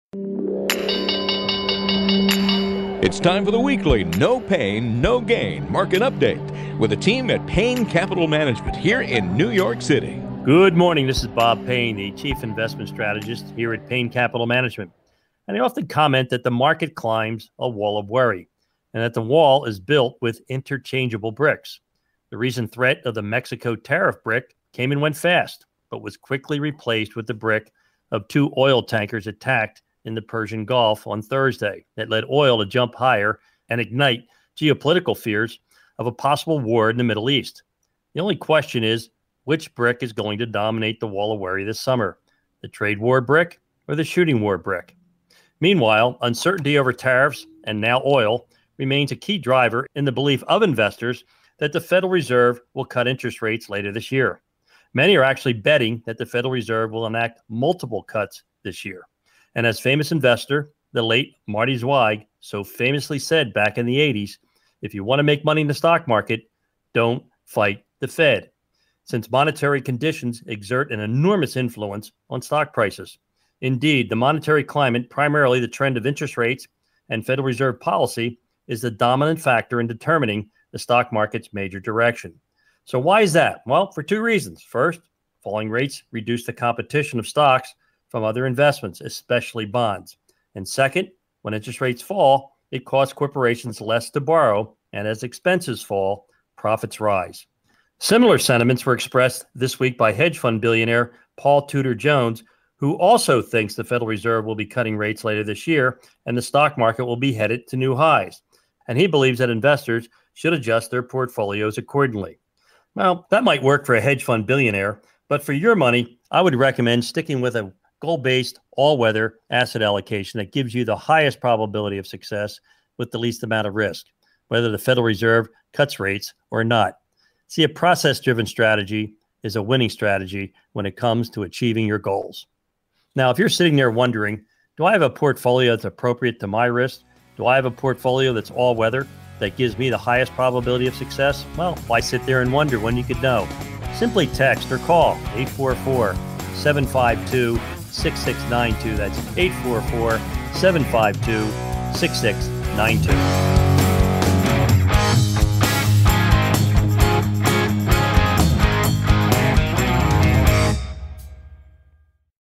2 oil tankers attacked in the Persian Gulf was the latest brick in the Wall of Worry
2 oil tankers attacked in the Persian Gulf was the latest brick in the Wall of Worry.

Will the fed to lower rates again this year?

Bob discusses worry in the market and the impact of Fed policies in this week’s Market Update.